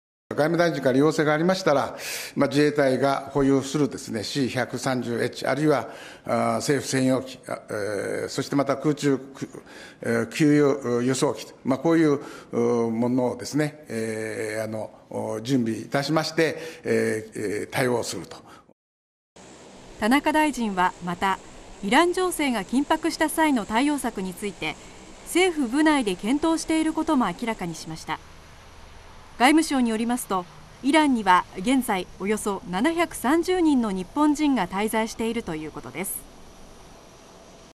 イラン有事の際、自衛隊機で邦人救出も 防衛大臣（12/03/05）
田中防衛大臣は、イランの核開発疑惑を巡って情勢が緊迫した場合、イランに在留する日本人の救出のため、自衛隊機の派遣を検討していることを明らかにしました。